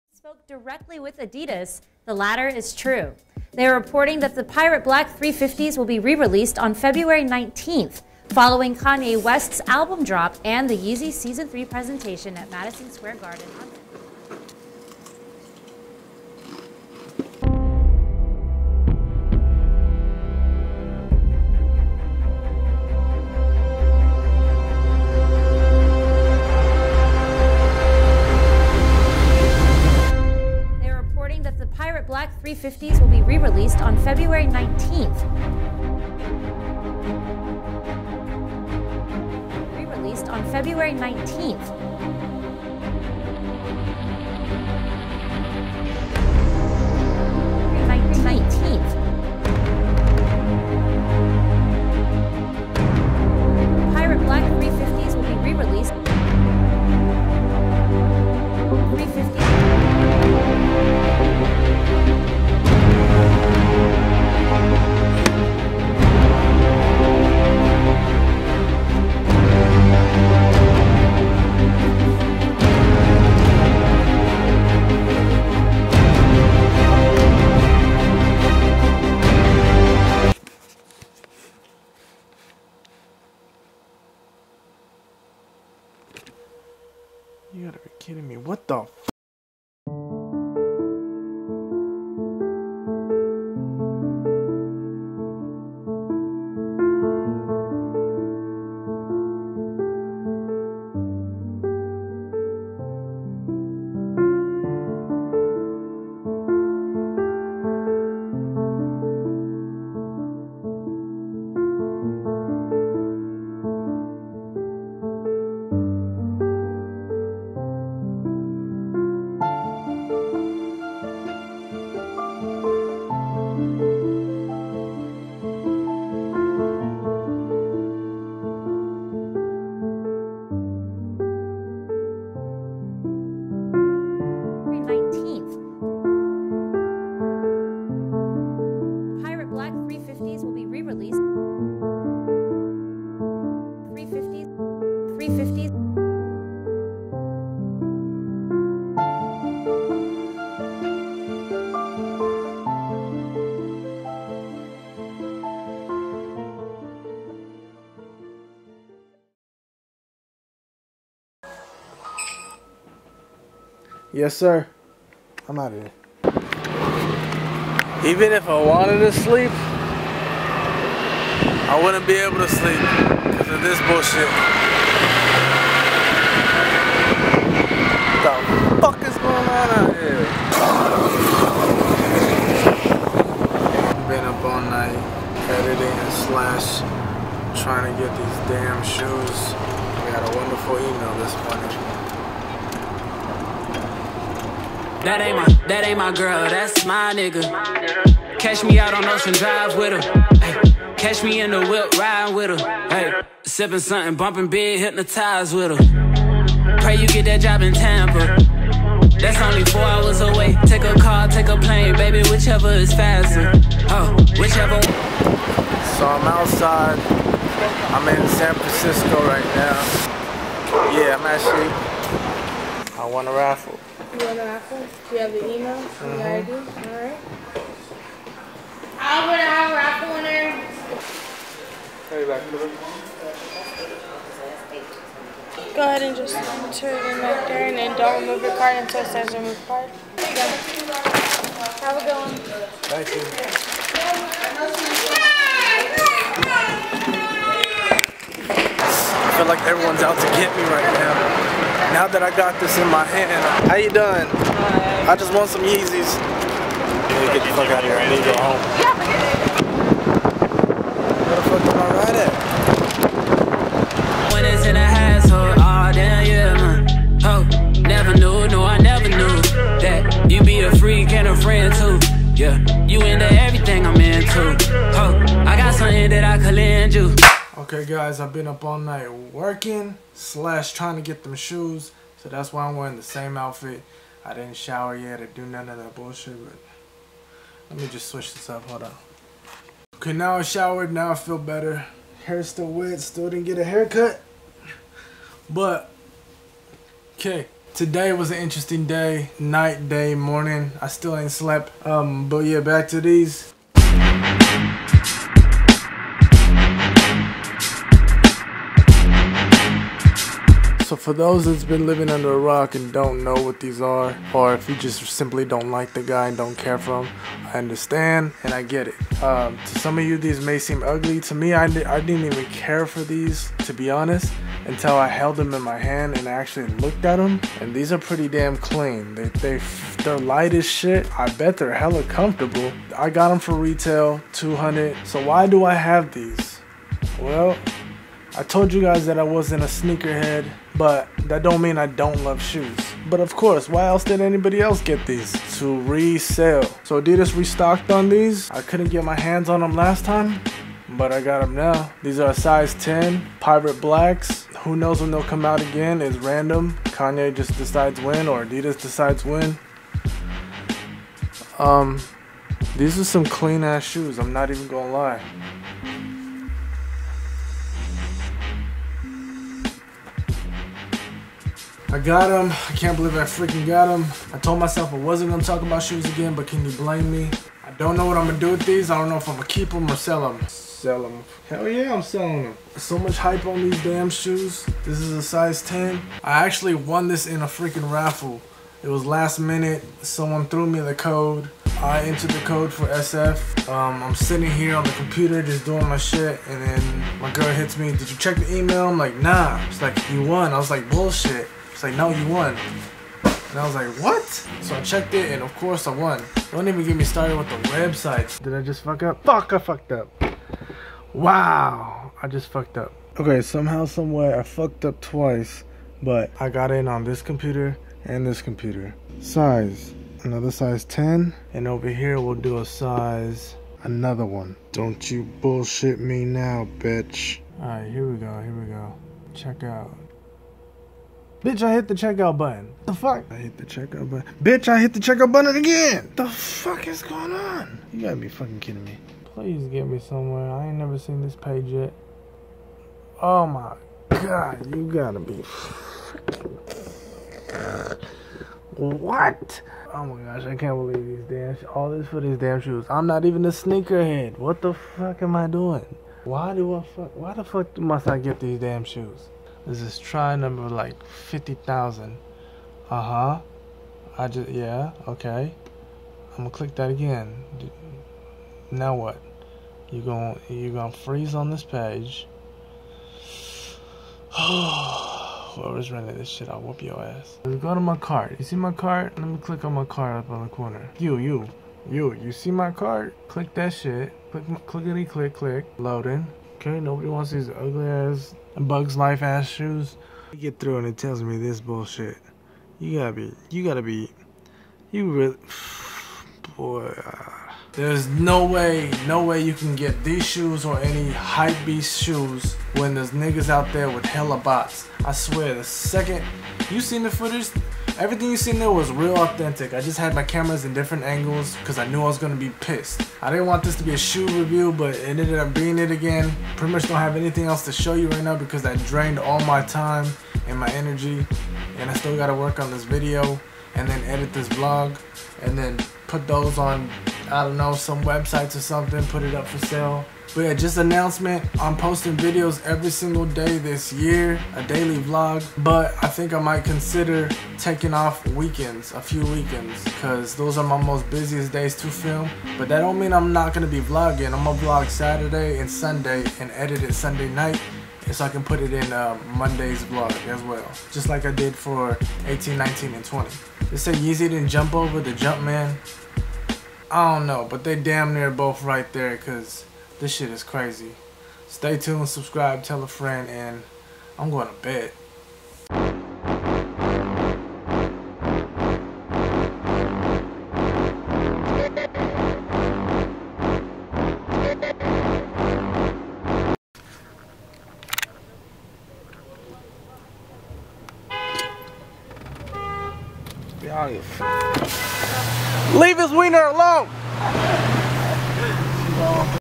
Trying to buy Yeezys MOST_VLOG_23
All I can say is, Im glad I didnt have to camp out for these. Never won a raffle in my life before.